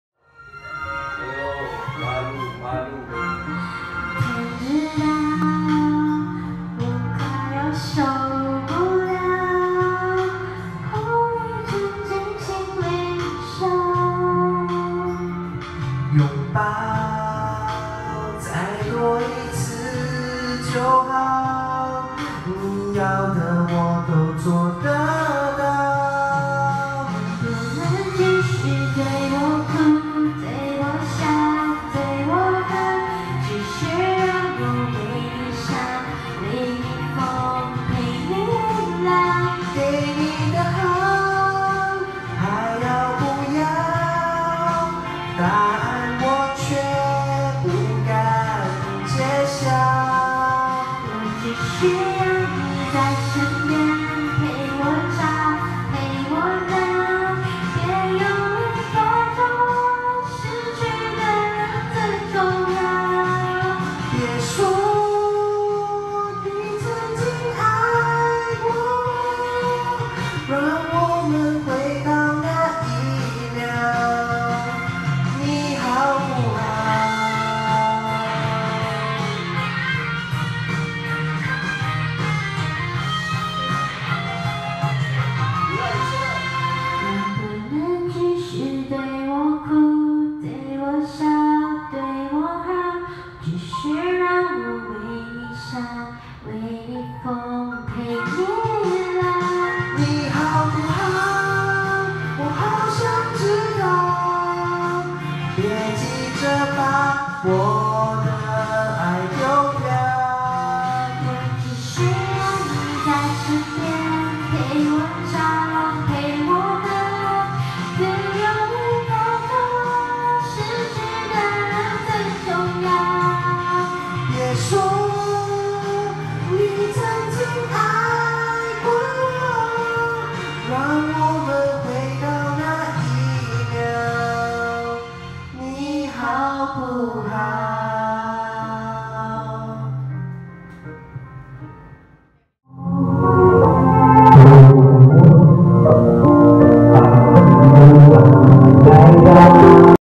{ Vlog 45 } Sing K session
Don't judge, sorry for the lousy quality and sound system, skipped to 3.40 :)
Recording for memories sake :'-)